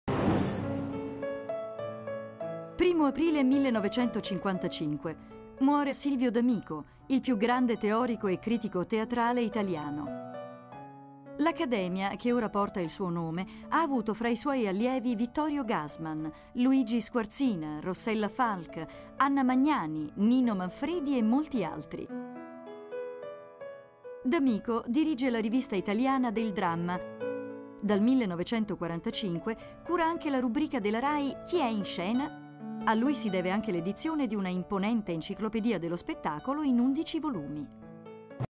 §.1/- (anniversari morte 1955) 01 APRILe, Roma: Silvio D’Amico, teorico e critico teatrale italiano
1955
:TITLE: Silvio D`Amico, teorico e critico teatrale
01/04/1955 Muore a Roma Silvio D’Amico, il più grande teorico e critico teatrale italiano. L'Accademia, che ora porta il suo nome, ha avuto tra i suoi allievi: Vittorio Gassman, Luigi Squarzina, Rossella Falk, Anna Magnani, Nino Manfredi e molti altri. D’Amico dirige la “Rivista italiana del Dramma”; dal 1945, cura anche la rubrica della Rai “Chi è in scena?”.A lui si deve anche l’edizione di una imponente Enciclopedia dello Spettacolo in 11 volumi.